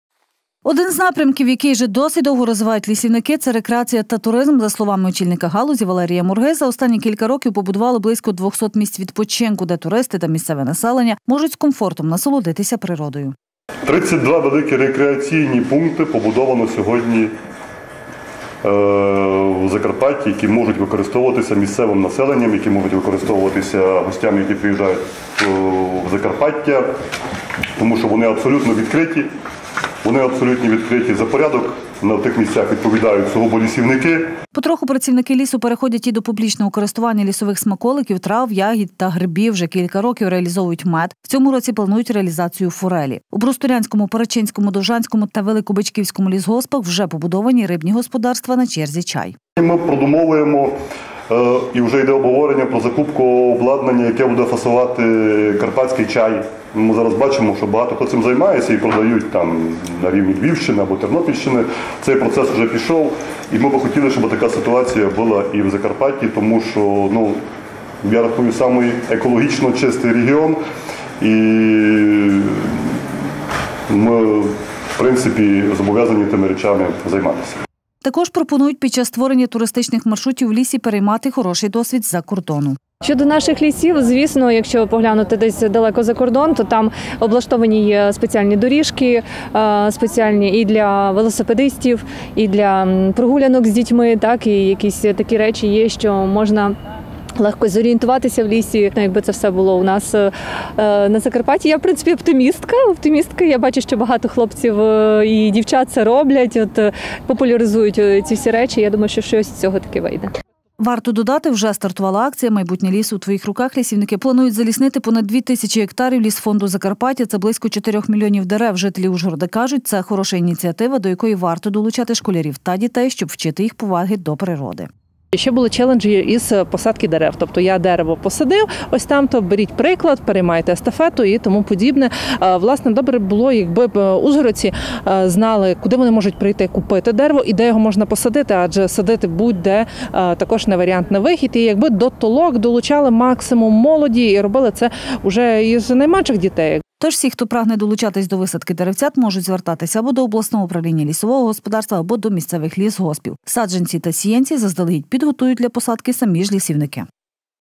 Рекреація та туризм на Закарпатті
Один із напрямків, який вже досить довго розвивають лісівники – це рекреація та туризм. За словами очільника галузі Валерія Мурги, за останні кілька років побудували близько двохсот місць відпочинку. Де туристи та місцеве населення можуть з комфортом насолодитися природою. Потроху працівники лісу переходять і до публічного користування лісових смаколиків – трав, ягід, грибів…Кілька років вже реалізовують мед, у цьому році планують реалізація форелі. Адже у Брустурнянському Перечинському, Довжанському та Великобичківському лісогоспах побудовані рибні господарства. На черзі – чай. Також ужгородці пропонують під час створення туристичних маршрутів у лісі переймати хороший досвід з-закордону. Варто додати, що вже стартувала акція «Майбутнє лісу у твоїх руках». Лісівники  планують заліснити понад 2,2 тис га лісфонду Закарпаття. А це близько 4 млн дерев. Жителі Ужгорода кажуть, це хороша ініціатива, до якої варто долучати школярів та дітей. Щоб вчити їх поваги до природи. Тож всі, хто хоче долучитись до висадки деревцят, можуть звертатися або до обласного управління лісового господарства, або до місцевих лісгоспів. Саджанці та сіянці заздалегідь підготують для посадки самі ж лісівники.